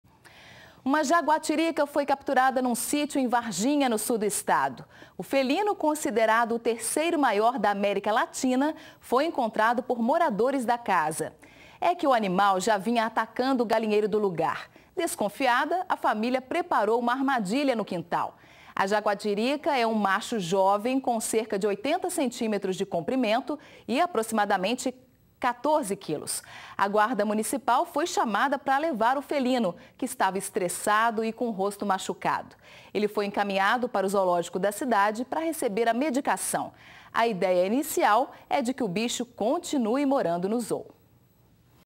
Jaguatirica é capturada em sítio no Sul de Minas
Uma jaguatirica foi capturada num sítio, em Varginha, no Sul do Estado. O felino, considerado o terceiro maior da América Latina, foi encontrado por moradores da casa.